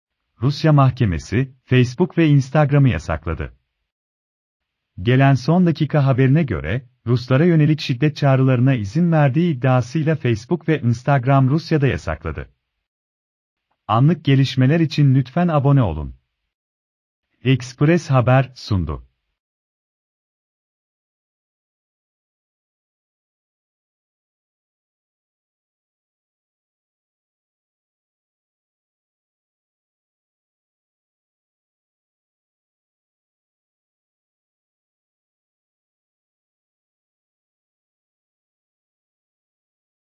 Son dakika! Rusya mahkemesi, Facebook ve Instagram’ı yasakladı
Gelen son dakika haberine göre, Ruslara yönelik şiddet çağrılarına izin verdiği iddiasıyla Facebook ve Instagram Rusya’da yasakladı